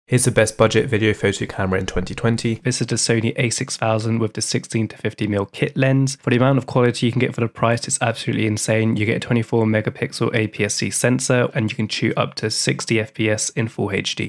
Best Budget Camera in 2020
What is the Best Budget Camera In 2020?

In my opinion, the best budget camera in 2020 is the Sony a6000. It is one of the best cameras I've used in that price range with amazing specs at an affordable price.